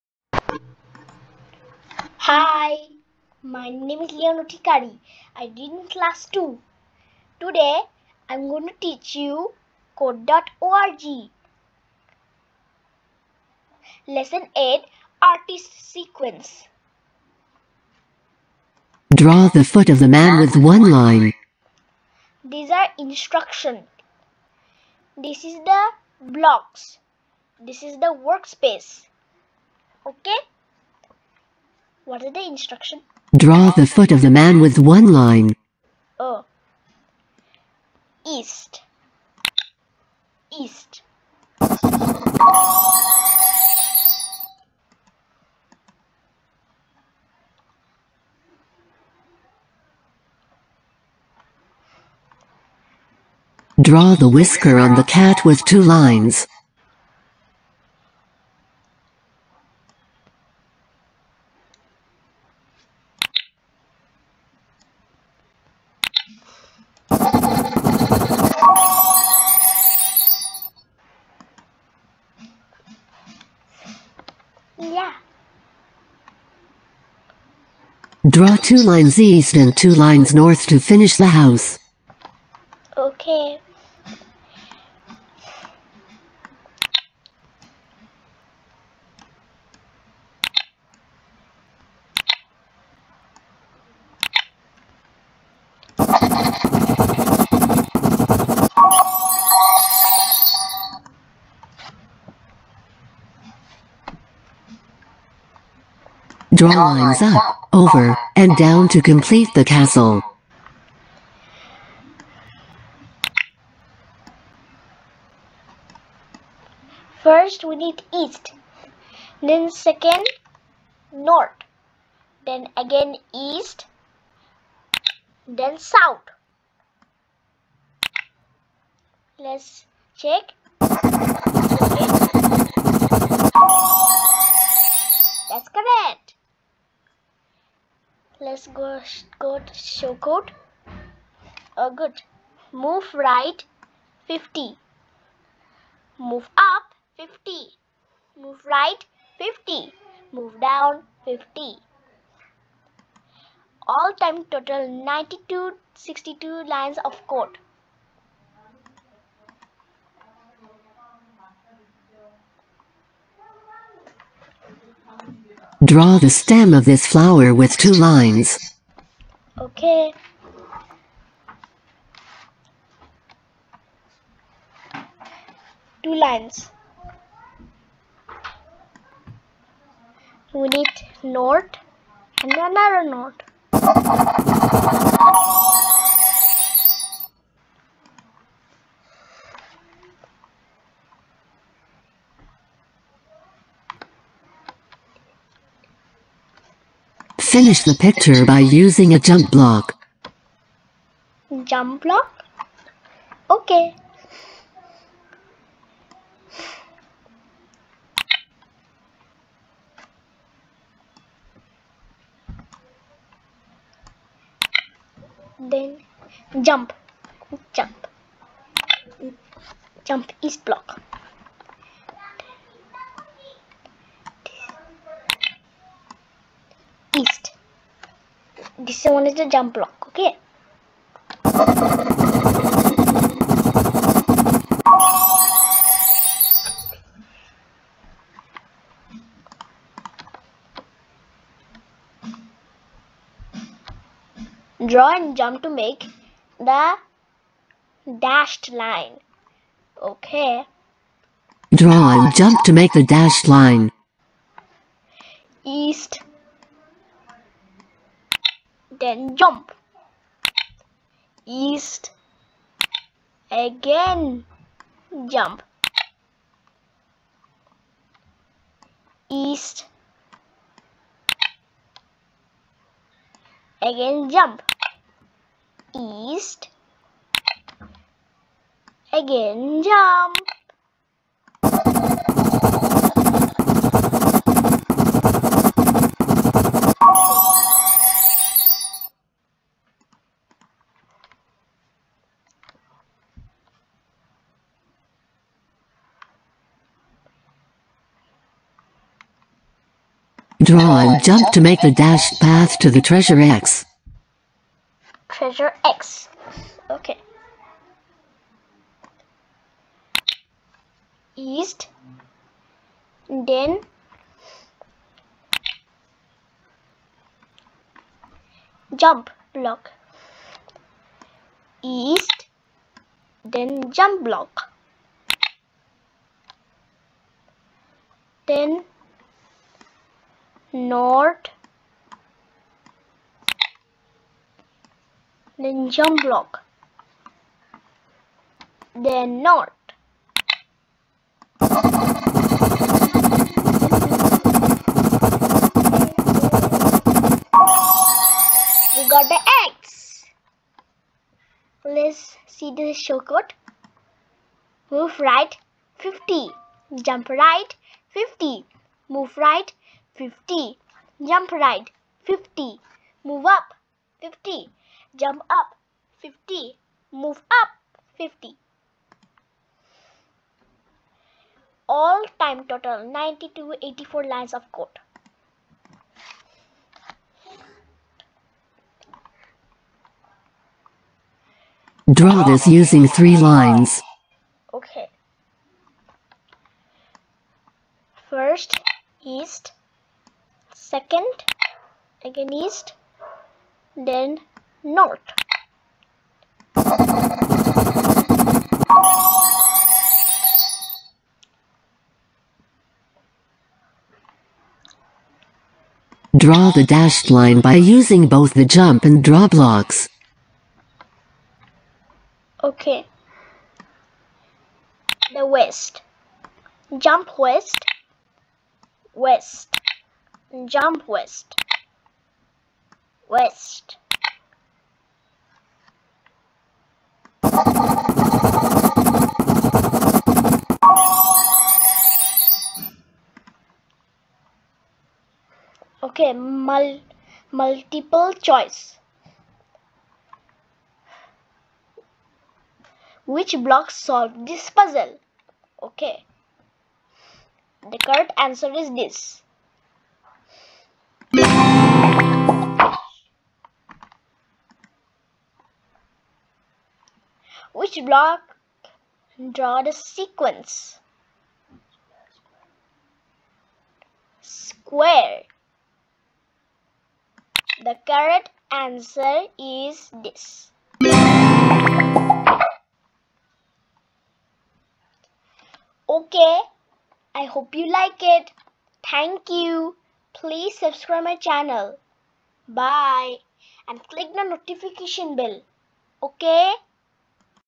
How to code using Artist Sequence #1 | Course 1 | Code org | Leon Adhikary
Hi Friends

In this video I have showed you how to use the code.org application. You can also learn this coding freely